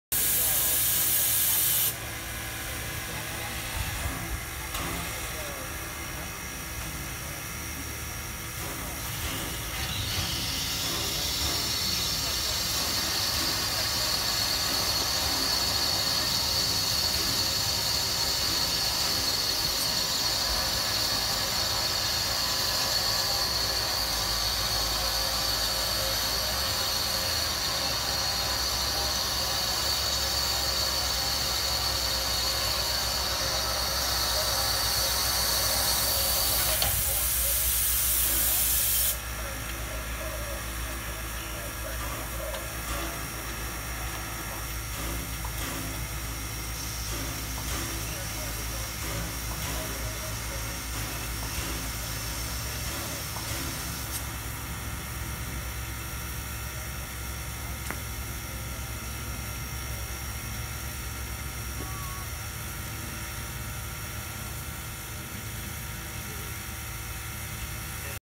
Frisco 1630 Waiting For Authority from Dispacher At the IRM
1630 is patiently waiting at the wye wating for authority from the dispatcher to go onto the mainline to retrieve his Coach train.

Steam Locomotive:
St. Louis-San Francisco (Frisco) 2-10-0 "Russian Decapod" class Ye No. 1630